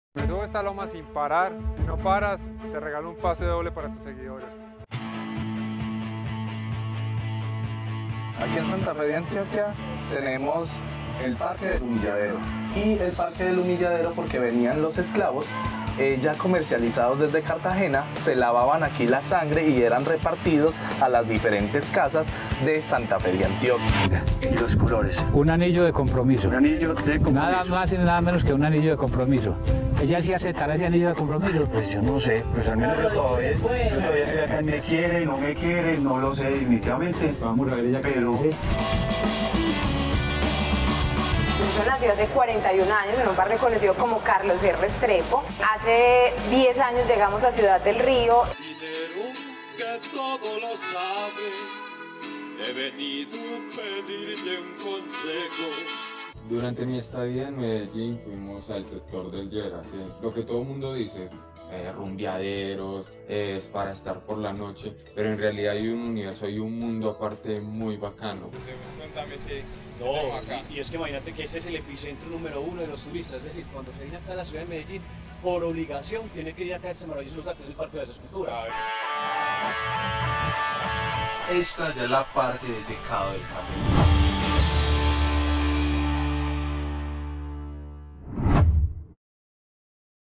Qué hay para hacer Trailer.
¡Sorpréndete viviendo diferentes experiencias que exploran lo que hay para hacer en Colombia!

Surprise yourself by living different experiences that explores what you can do in Colombia.